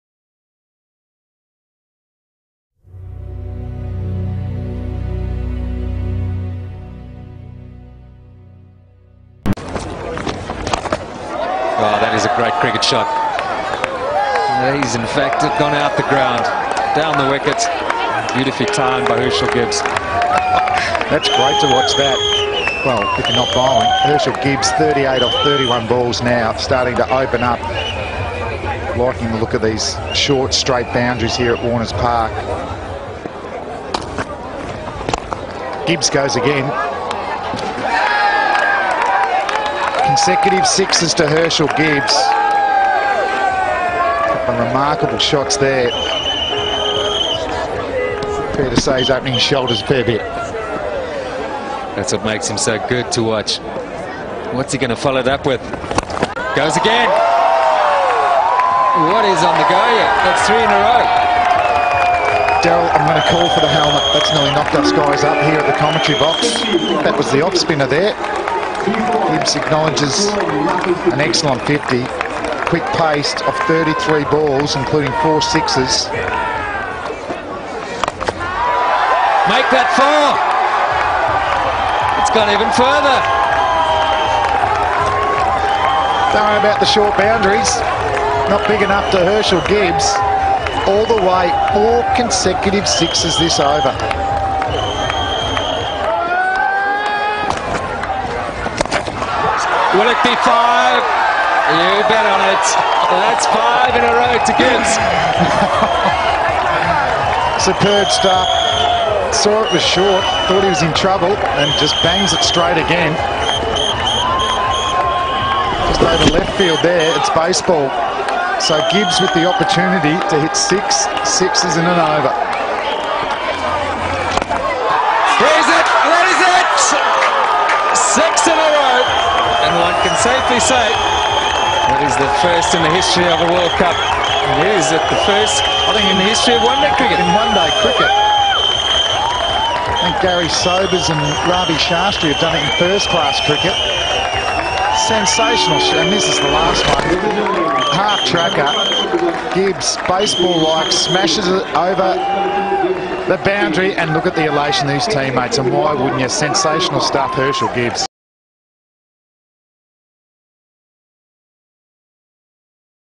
Herschelle Gibbs six 6's In One Over | Cricket records | Most expensive over in cricket history
Herschelle Gibbs hits six 6's in one over against netherlands in world cup 2007